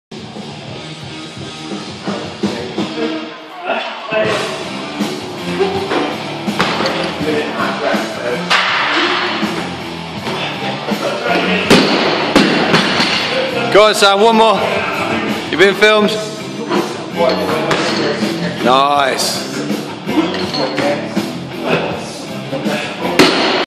Saturday Morning PTB'ers & Lifting Practice
The 12-week PTB consists of 2 resistance workout sessions and 1-2 conditioning session with an optional home core workout. However we also spend time in the gym PRACTISING what we love. We're still lifting weights but in a recreational, skill developmental way. The 12-week PTB contains your EXERCISE but make sure you do other things you enjoy for RECREATION for fun, social, practice, passion etc.